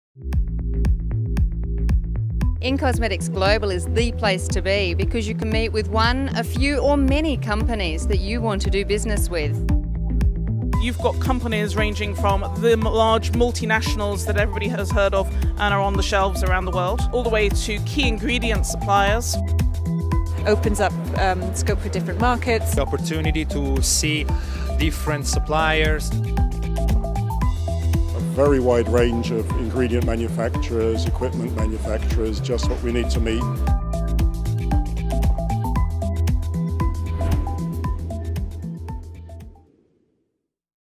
Targeting the right exhibitors for you at in-cosmetics Global
From large suppliers, to small niche suppliers, in-cosmetics had it all. But how do you know which ones are right for you? Discover what's in-store for you at in-cosmetics Global 2020.